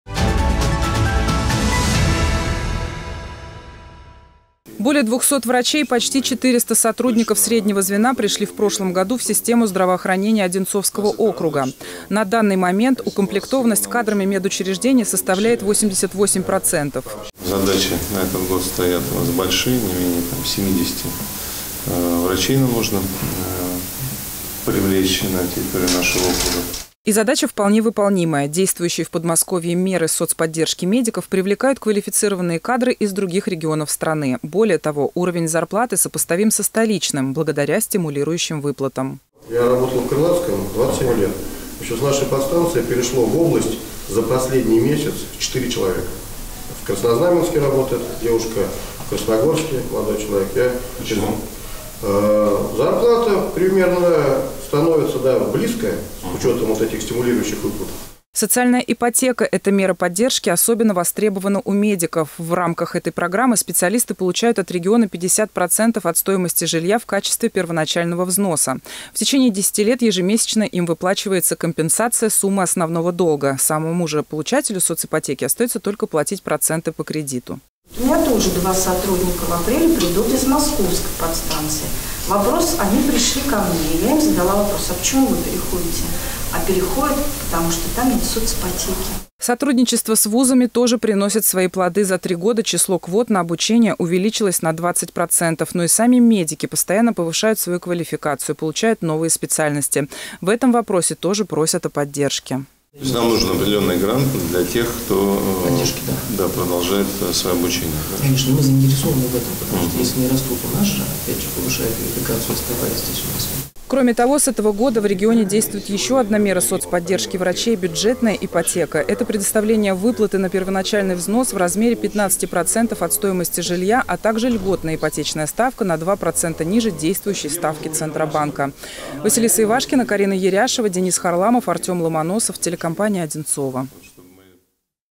Кадры решают всё
Более 200 врачей, почти 400 сотрудников среднего звена пришли на работу в систему здравоохранения округа. О мерах поддержки медиков – в материале нашего корреспондента.